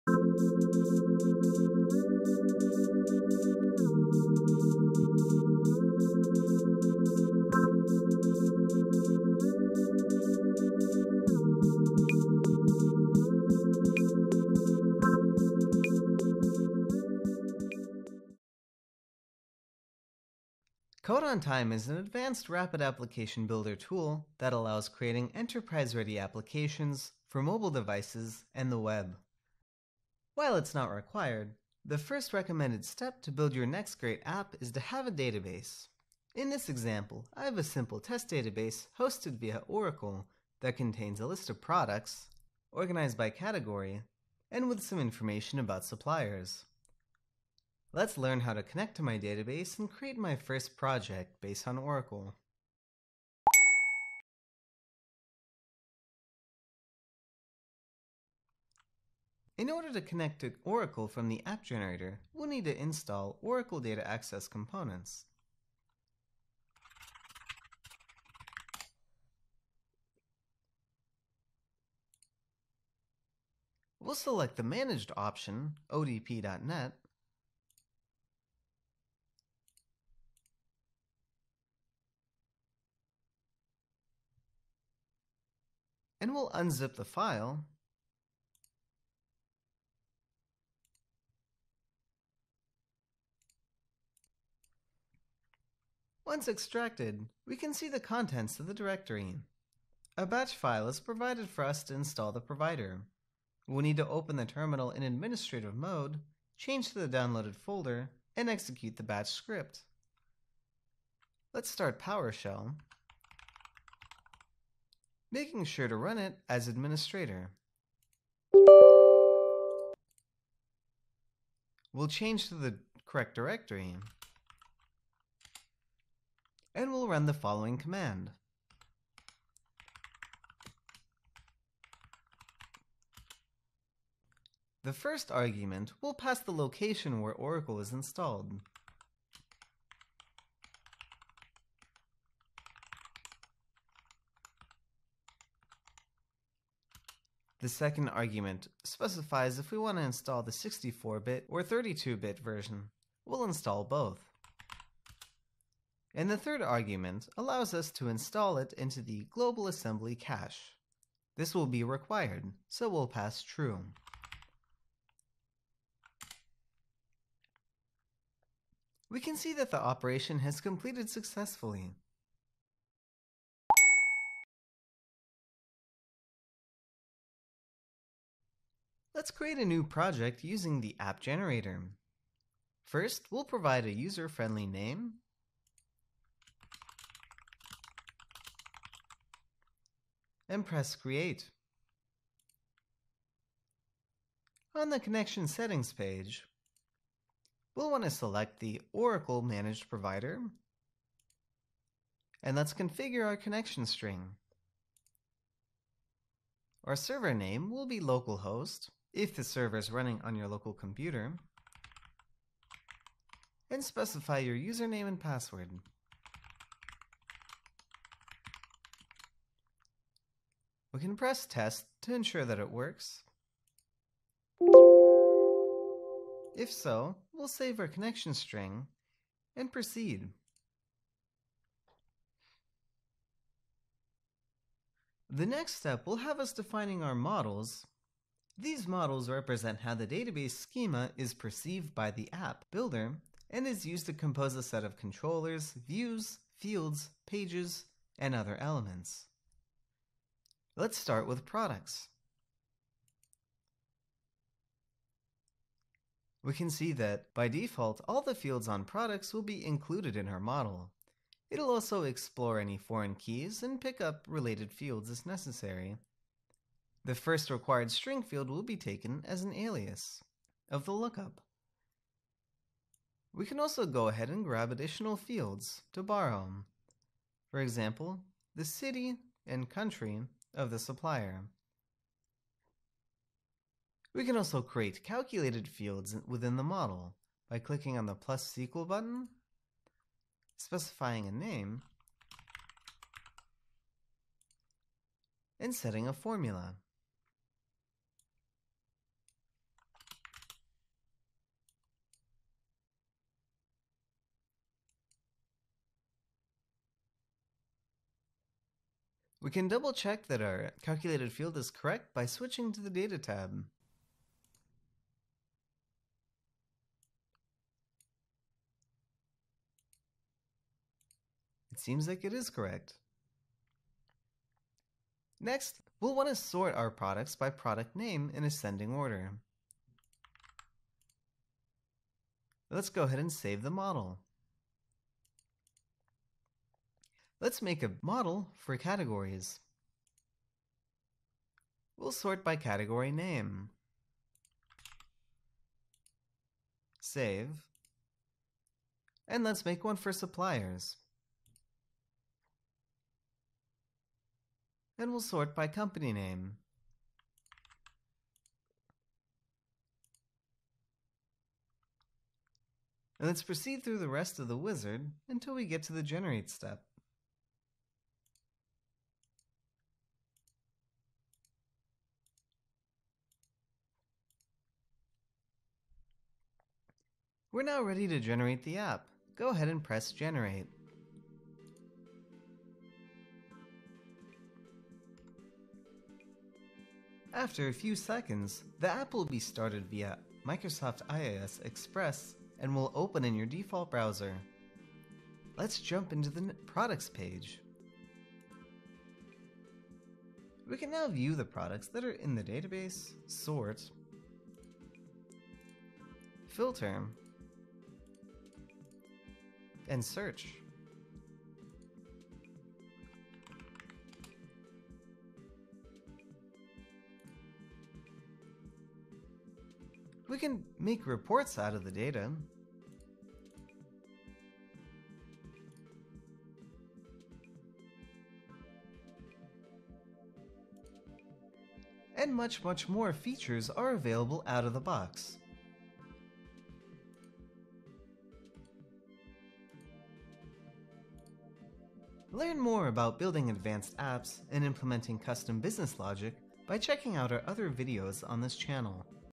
Oracle and Low Code App Development
Learn to create online and offline database apps with Oracle and Code On Time, low code rapid application development tool.